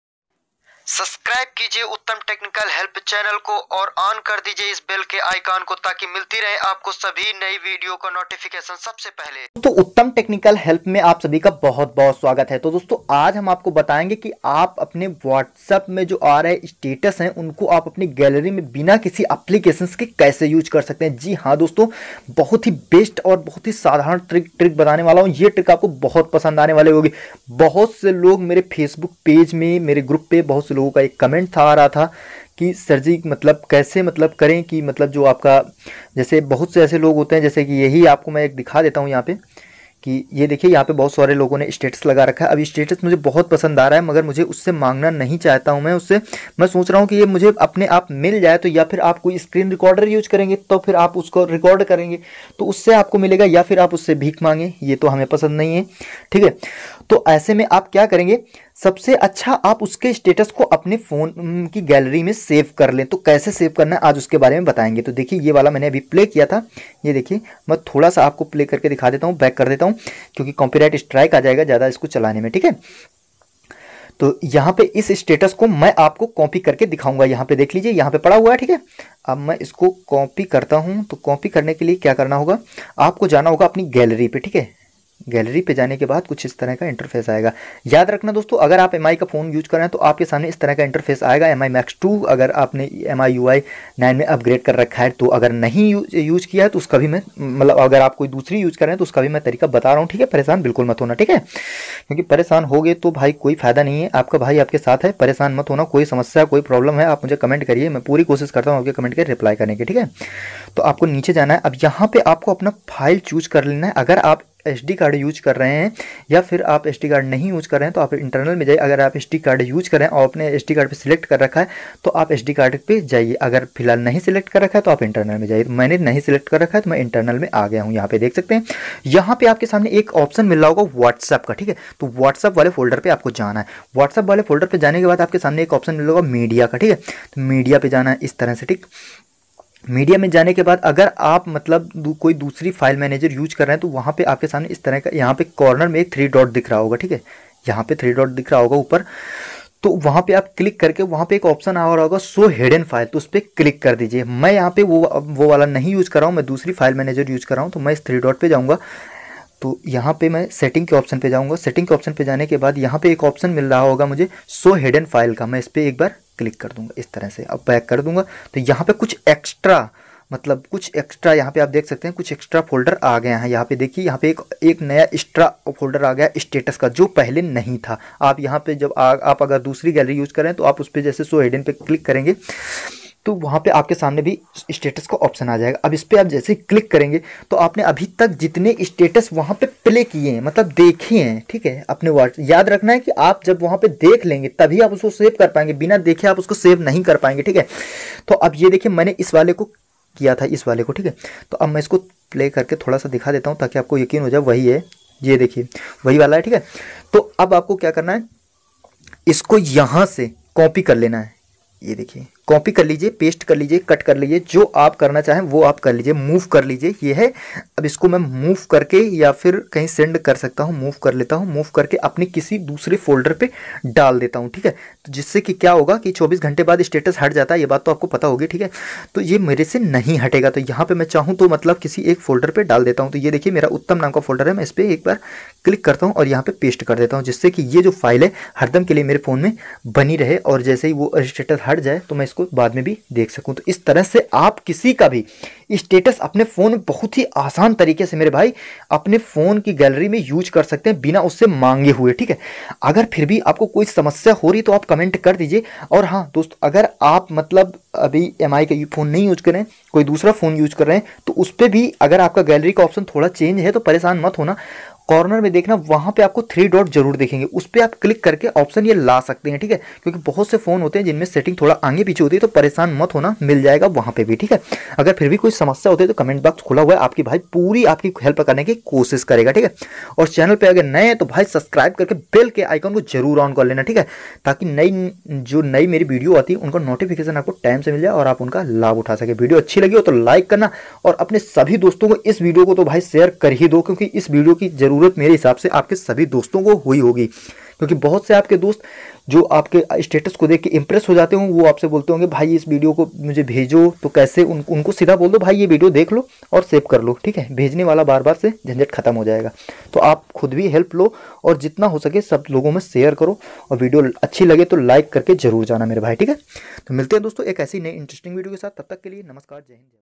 how to save whatsapp status video,WhatsApp status phone ki gallery me kaise save kare, status save,h
Hi

About this video- Dosto aj ham apko batayege ki kis tarah se ap apne WhatsApp status ko apni gallery me save kar sakte hai